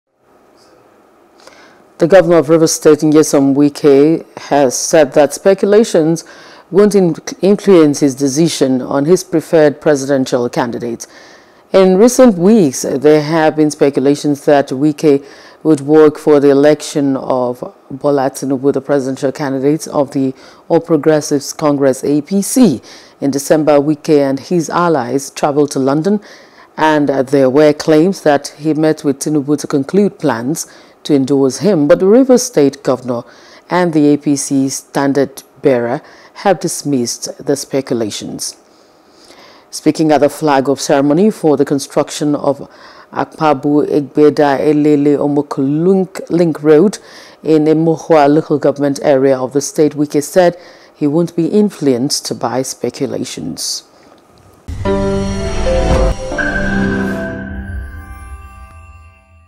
Wike: Speculations Won’t Influence My Choice Of Presidential Candidate
Governor of Rivers, Nyesom Wike, says speculations won’t influence his decision on his preferred presidential candidate.